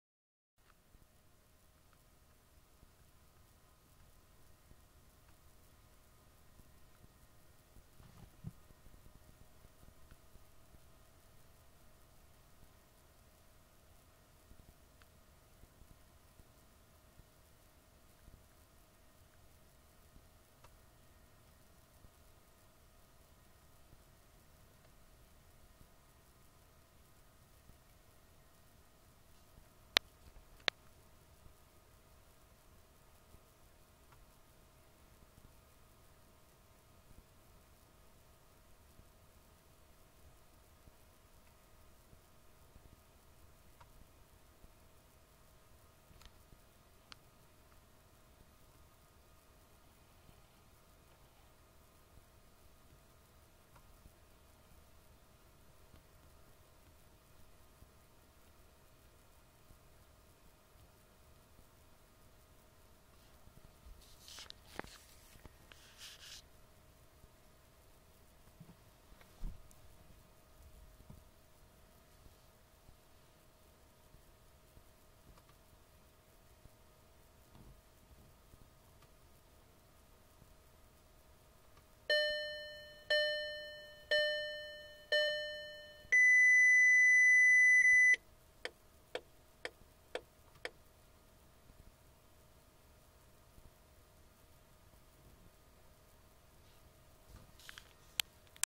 Passat 3C Instrument Cluster Test
Instrument Cluster Test - Passat 3C. Just a test of the Instrument Cluster in a Passat 3C.